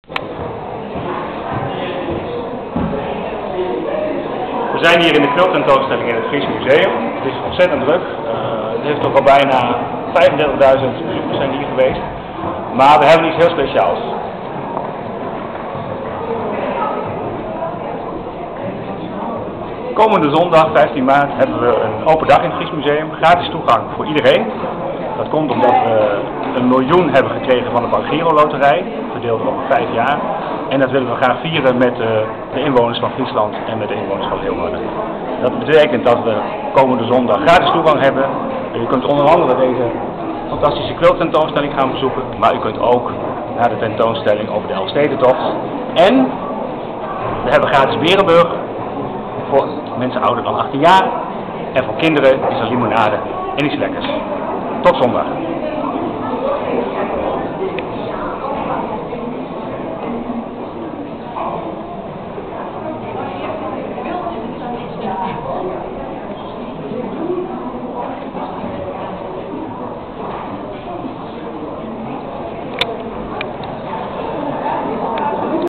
Open Dag Fries Museum
Het Fries Museum heeft iets te vieren en houdt daarom op zondag 15 maart een open dag. Van 11.00 tot 17.00 uur is de toegang gratis voor iedereen. Begin februari is bekendgemaakt dat het museum in de komende vijf jaar jaarlijks een bedrag van 200.000 euro ontvangt van de BankGiro Loterij. Dat geweldige nieuws wil het Fries Museum vieren met de bezoekers en inwoners van Friesland. Daarom staan de deuren op zondag 15 maart open. Er is muziek van Accordéon Mélancholique en gratis Sonnema Berenburg.

Het Fries Museum nodigt u van harte uit om op 15 maart de verschillende tentoonstellingen te komen bekijken. Kom kijken bij de kleurrijke hoofdtentoonstelling Quilts - Kunst met een Q die inmiddels al meer dan 30.000 bezoekers heeft getrokken, maar bezoek ook de presentatie Elfstedentocht in Oorlogstijd, die het verhaal vertelt van de Elfstedentochten die in 1940, 1941 en 1942 zijn verreden. Buro Leeuwarden presenteert het nieuwste werk van Pedro Bakker. In de tentoonstelling The End in Kabinet Knecht-Drenth is onder meer werk te zien van Armando, John Baldessari en Marlene Dumas. In De Verzamelplaats presenteert de 7-jarige Emke Sijtsma haar verzameling over paarden. Maar u kunt natuurlijk ook de Mata Hari-zaal gaan bezoeken, de 16- en 17de-eeuwse Friese portretten of het schitterende Fries zilver.

Bezoekers kunnen kennismaken met de nieuwe directeur van het Fries Museum, Saskia Bak. Van 13.30 tot 16.30 speelt het duo Accordéon Mélancholique - Chérie de Boer en Jean-Pierre Guiran - wereldmuziek in het museum. In het museumcafé is er voor iedere bezoeker boven de 18 gratis berenburg, mogelijk gemaakt door Sonnema Berenburg uit Bolsward. Voor kinderen is er limonade en iets lekkers.

Het Fries Museum is enorm blij met de honorering van de BankGiro Loterij, de cultuurloterij van Nederland. Het museum gaat volgens aanvraag het geld inzetten voor inrichting en eerste nieuwe presentaties van het nieuw Fries Museum. Eind 2012 opent het nieuwe museum zijn deuren voor het publiek.